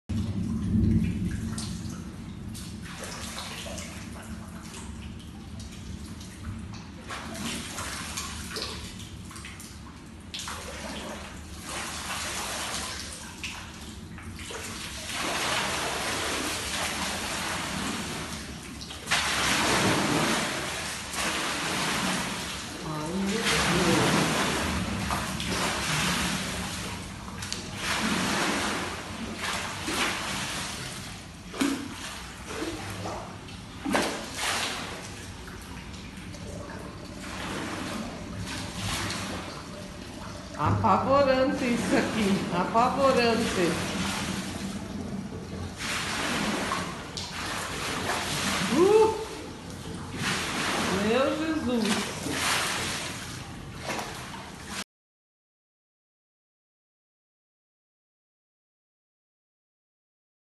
Arranha-céu balança durante tempestade em Balneário Camboriú
Imagens mostram o balanço da água dentro da piscina de um apartamento